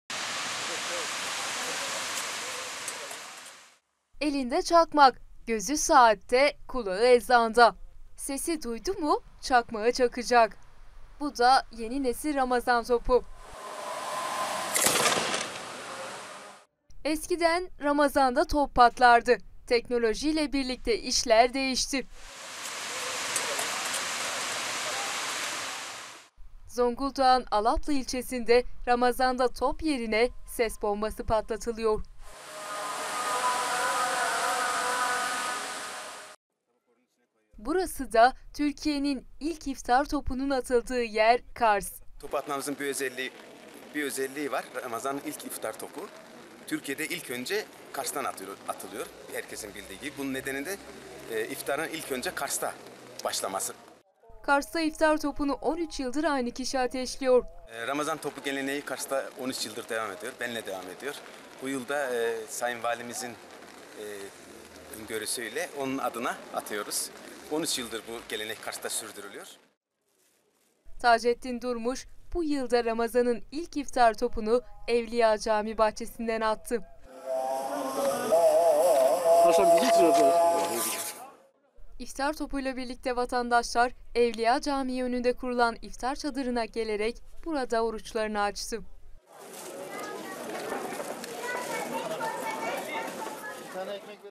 Ramazan Topu Geleneği Yaşatılıyor
Ramazan toplarının yerini gelişen teknoloji ile birlikte ses bombaları aldı ama gelenekten vazgeçilmedi.

Alaplı Belediye Başkanı Nuri Tekin, yaptığı açıklamada, uzun zamandır süregelen güzel bir geleneği yaşatmaya çalıştıklarını, piyasada kara barut bulmakta güçlük çektikleri için, iftar vakitlerini bildirmek üzere eski kuru sıkı top atışı geleneğini terk etmek zorunda kaldıklarını söyledi. Tekin, top atışı yerine Ramazan boyunca iftar vakitlerini duyurmak için ses bombası kullandıklarını dile getirdi. Tekin, "Osmanlı’dan günümüze kadar Ramazanlar’da iftar ve sahur vakitlerini duyurmak için haber vermek için eskiden dökümden yapılmış savaş topları patlatılıyordu. Teknolojinin gelişmesi ile artık belediyelerde bu gibi hizmetleri ses bombaları ile patlatarak nostaljik geleneği yaşatmaya çalışıyoruz. Alaplı’nın çoğu kesimi, top sesi ile iftar açıyor. Zabıta memurumuz tarafından iftar vakti ses bombası patlatılarak güzel geleneğimizi ses bombasıyla da olsa yaşatmaya çalışıyoruz. Ramazan gelenekleri ve adetleri her yerde olduğu gibi ilçemizde de devam ediyor. 1900 yıllarda iftar saatlerinde ramazan topu atışı iskeleden gerçekleştiriliyordu. Bildiğimiz top atışı olurdu.12 Eylülden sonra top atışana ara verildi. 1990 yıllarda teknolojinin gelişmesiyle birlikte ses bombaları çıktı. Bugüne kadarda iftar saatinde dolgu alanından ya da Atatürk parkından zabıta ekipleri tarafından ezanın okunmasıyla birlikte ses bombası patlatılıyor. İlçemizde iftarını top sesiyle açmak isteyen vatandaşlar, iftar saatinde kapı ve pencerelerde bekliyorlar" dedi.

KARS

Kars Valiliğince 13 yıldır geleneksel hale getirilen “Türkiye’de ilk iftar topu”nun atıldığı yer olan Kars’ta gelen değişmedi. 13 yıldır iftar topunu ateşleyen Tacettin Durmuş, bu yılda Ramazan’ın ilk iftar topunu Evliya Cami bahçesinden attı. Kars Valiliğince 13 yıldır iftar topunun geleneksel hale getirildiğini ifade eden Durmuş, “Ramazan topu geleneği 13 yıldır Kars’ta devam ediyor. Bu yılda Sayın Valimizin ön görüsüyle atıyoruz. 13 yıldır bu gelenek Kars’ta sürdürülüyor. Ebul Hasan Harakani Türbesi’nin bulunduğu manevi mekandayık. Ramazan topu atmamızın bir özelliği var. Ramazanın ilk iftar topu Türkiye’de ilk önce Kars’tan atılıyor” dedi.

İftar topuyla birlikte vatandaşlar Evliya Cami önünde kurulan iftar çadırına gelerek burada oruçlarını açtılar.